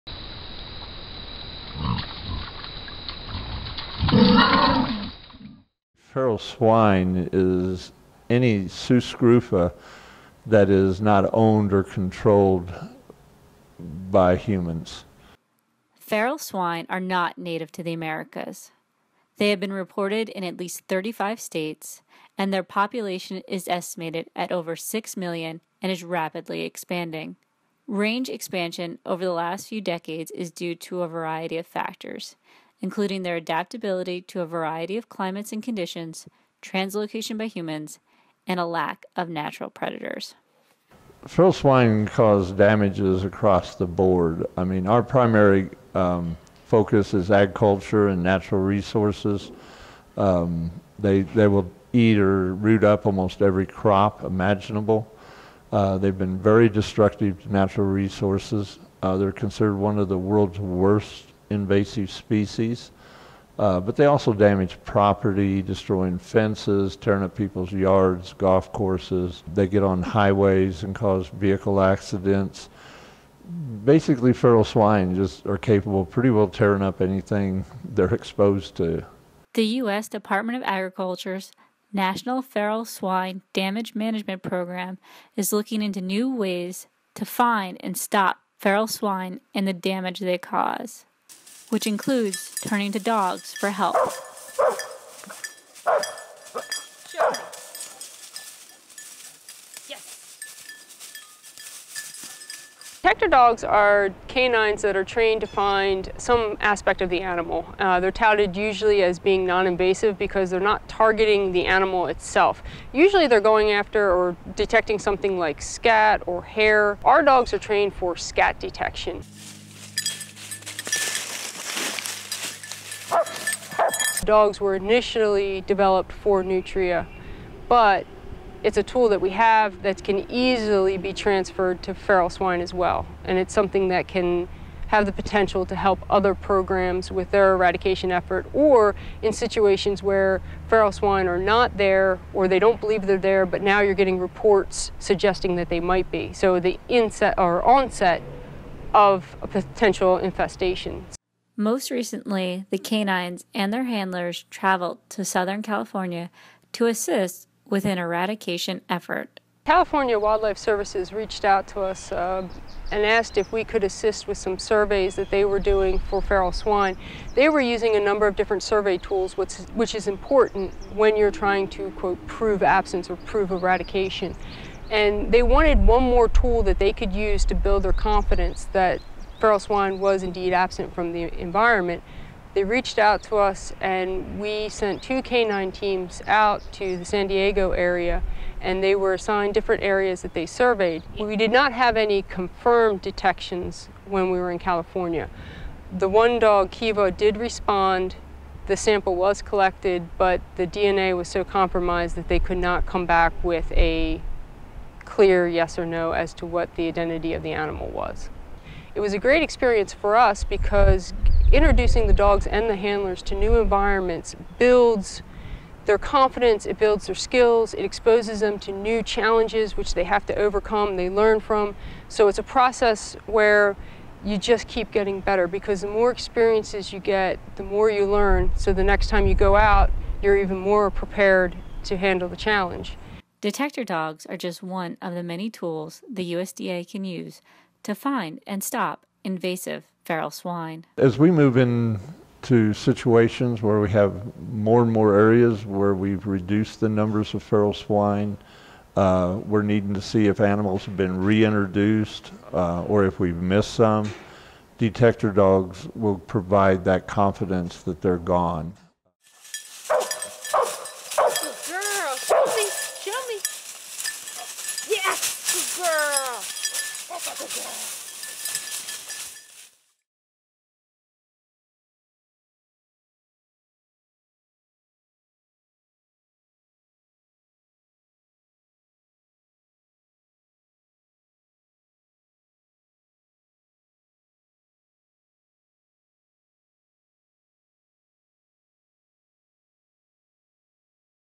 USDA Unleashes Detector Dogs to Stop Feral Swine?
USDA Wildlife Services in Maryland recently added scent-detector dogs to the array of tools it uses to assist partners in protection of the Chesapeake Bay region through eradication of the invasive rodent, the nutria.  As nutria populations decline finding them in the landscape became more challenging but dogs trained to detect nutria scat are helping to confirm the presence or absence of nutria.  The team is now cross-training the canines to address the challenge of invasive feral swine.